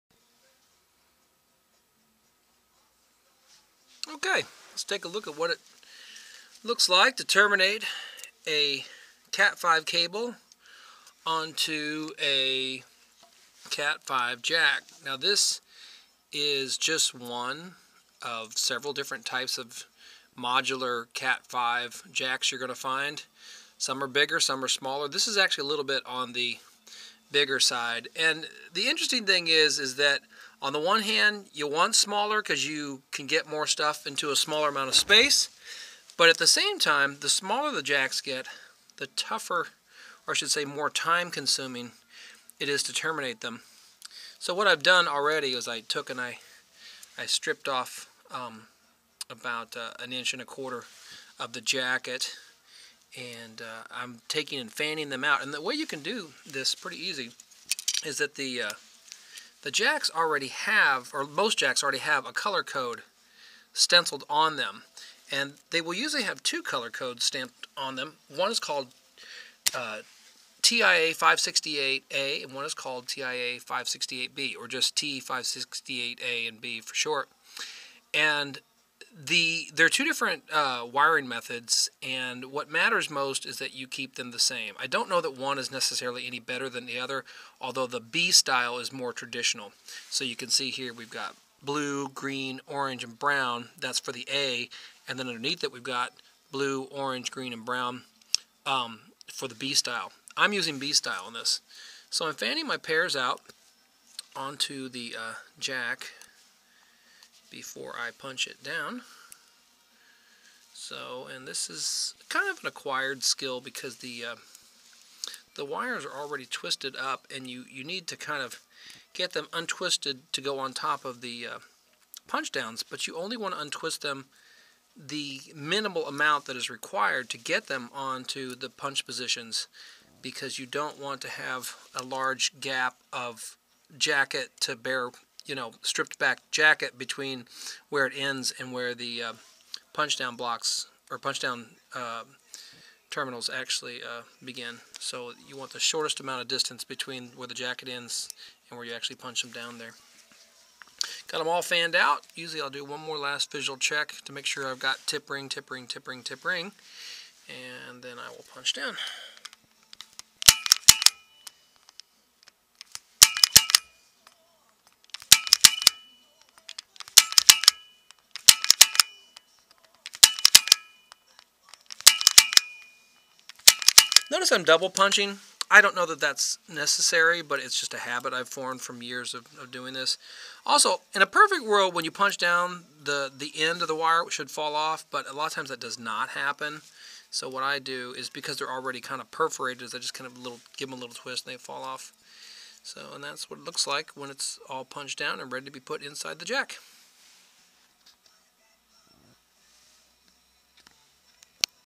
wiring cat5e jack
Shows an Ortronics cat5e jack being terminated (connected) to a category 5e 8-wire cable.  The same technique could be used for a category 6 cable as well.